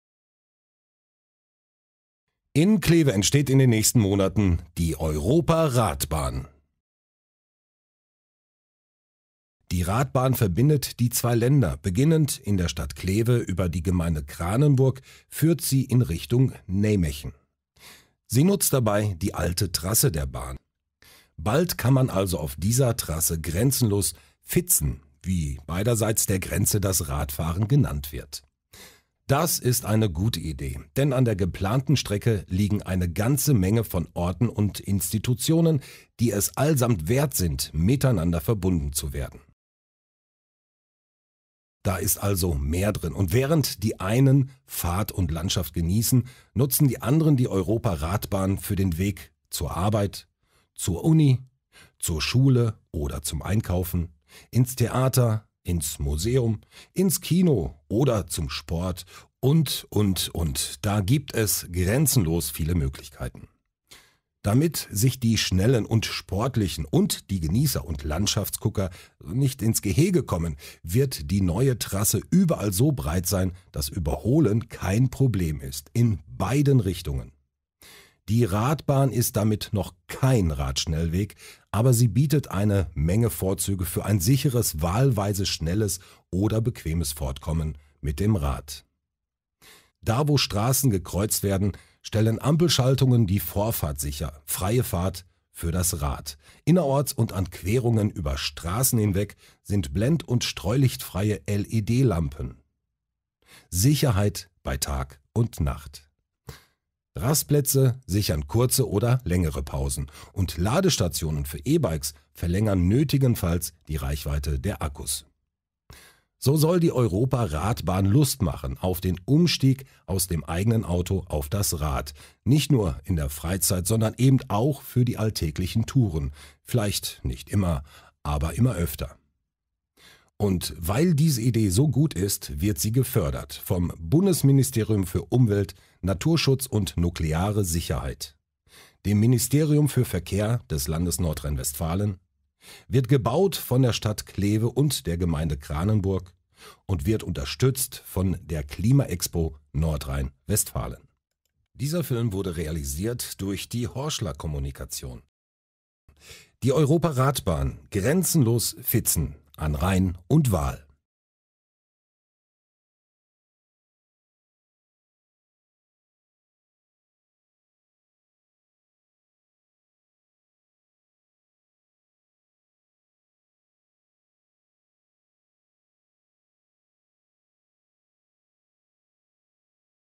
EuropaRadbahn Kleve - Kranenburg
Die Europa-Radbahn: Eine Einladung zum Radfahren

Moderne (Fahrrad-)Technik und ein steigendes Umweltbewusstsein, ein sich veränderndes Mobilitätsverhalten und die wachsende Bereitschaft das eigene Auto auch mal stehen zu lassen sind gute Bedingungen für den Umstieg auf das Fahrrad auch im Alltag: Ein wichtiger Beitrag zur eigenen Gesundheit und zur klimafreundlichen Fortbewegung.